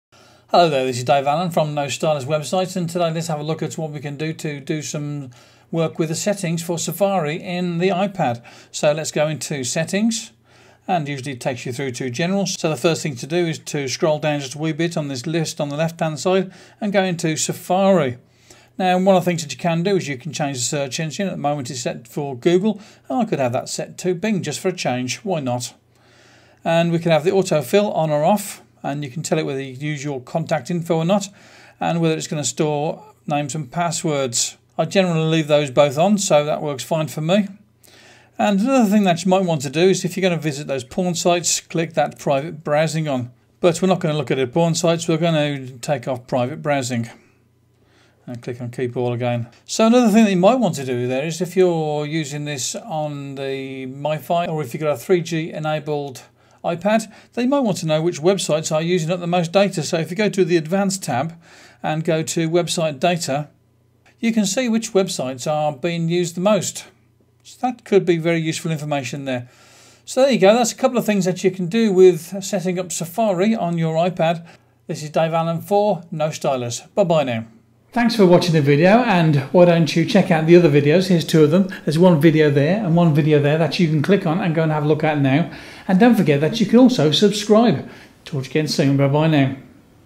Safari Settings on iPad
I suppose there could be other reasons for wanting to use private browsing rather than porn, but it something that you can set up easily on the iPad in the system settings